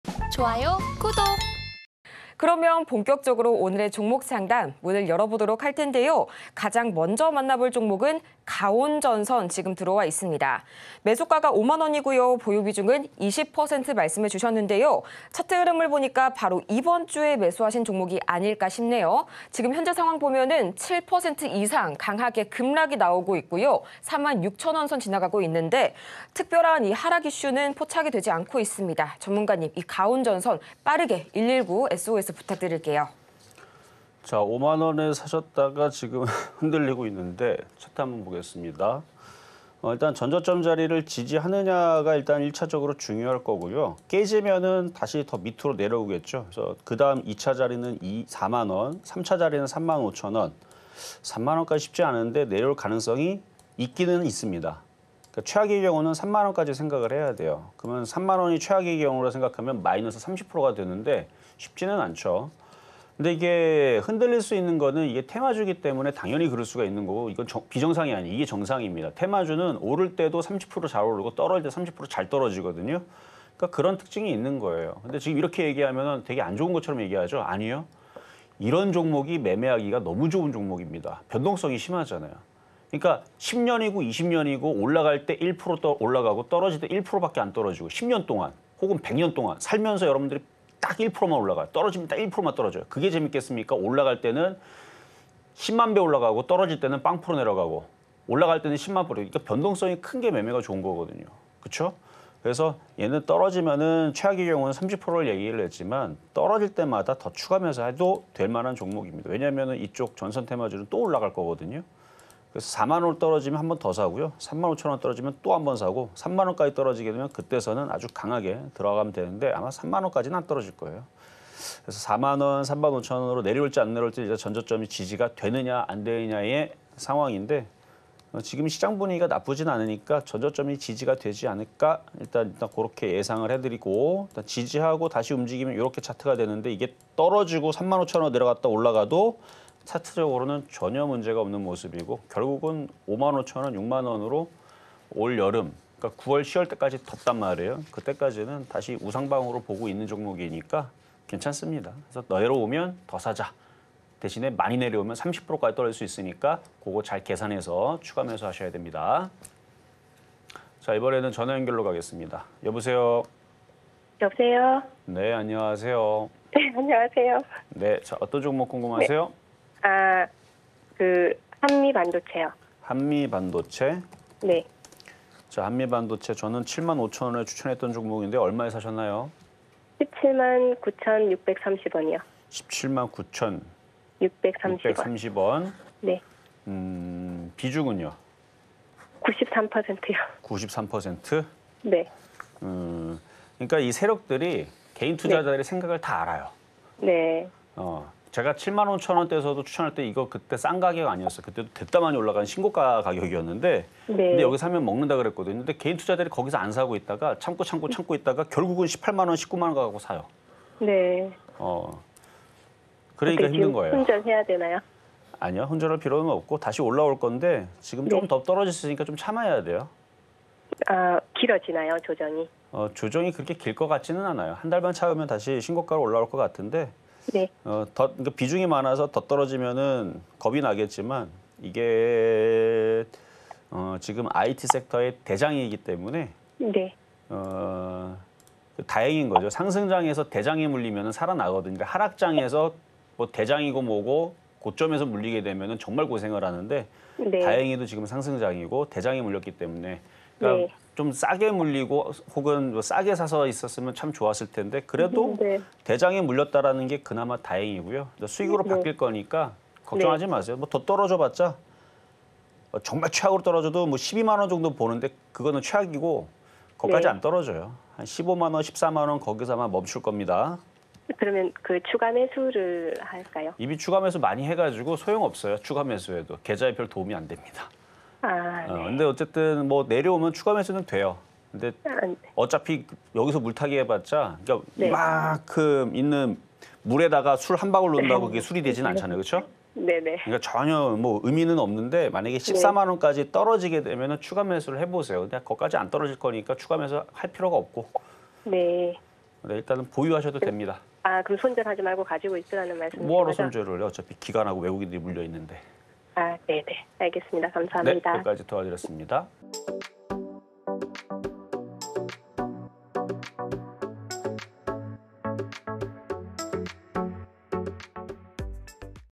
[종목상담] 가온전선, 한미반도체 종목 상담 부탁드려요! │ 주식, 주가
가온전선 (000500)  매수가: 50,000원 / 비중: 20%
한미반도체 (042700) 메수가: 179,000원 / 비중: 93%

자세한 내용은 [실전매매의 정수] 영상을 통해 확인하시기 바랍니다.